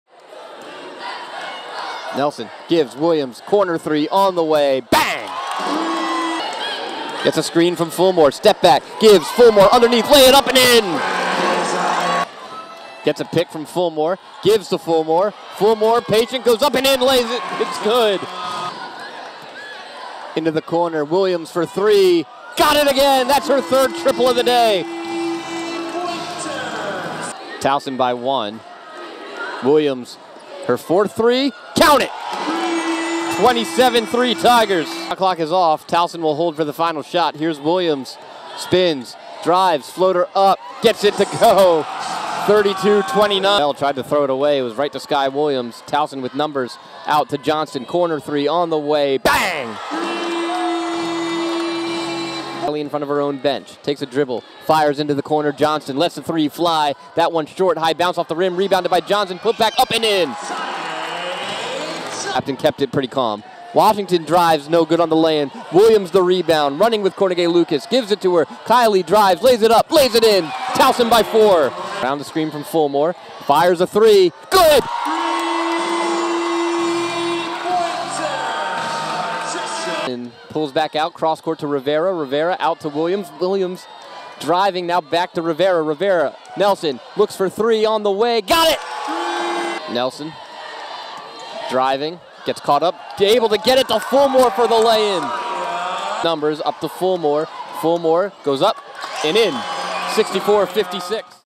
Towson Women's Basketball Take Down The Drexel Dragons 70-61
The Towson Women's Basketball team returned to SECU Arena on Sunday and took down the Drexel Dragons to keep their five game win streak alive.